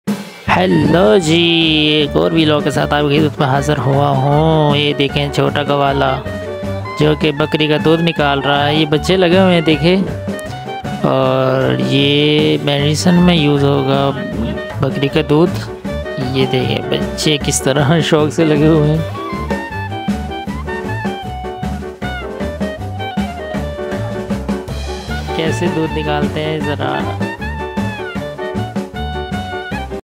Goat milking in the village | Goat Milk | naughty child | Bakri ka doodh nikalna
In this video naughty childrens are going to milking by hand.